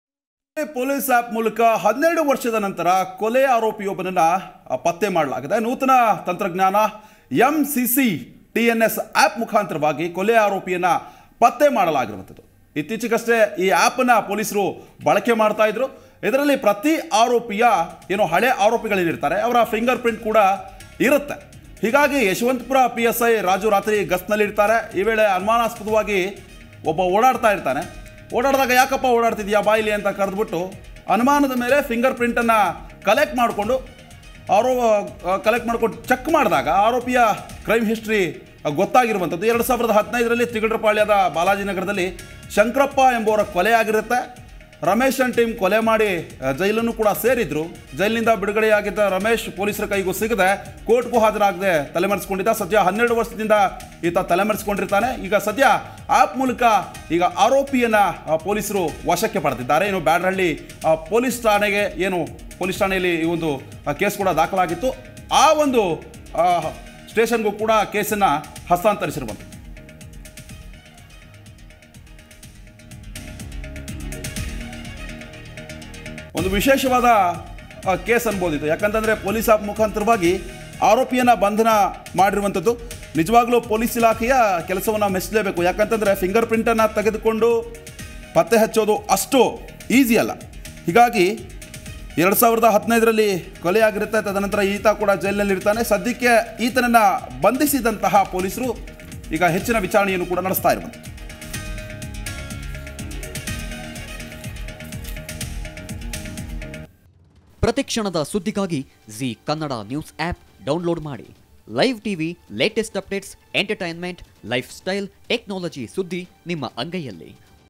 Police | Yeswanthpur |ಪೊಲೀಸ್‌ app ಮೂಲಕ ಬೆಂಗಳೂರು ಪೊಲೀಸ್‌ ಸಾಧನೆ
ಯಶವಂತಪುರ ಪೊಲೀಸರ ಕಾರ್ಯಕ್ಕೆ ಮೆಚ್ಚುಗೆ
ಫಿಂಗರ್‌ ಪ್ರಿಂಟ್‌ ವೇಳೆ 2015ರ ಕೊಲೆ ಕೇಸ್‌ ಪತ್ತೆ
ಪೊಲೀಸರಿಗೆ ಸಿಗದೆ, ಕೋರ್ಟ್‌ಗೆ ಹಾಜರಾಗದ ಆರೋಪಿ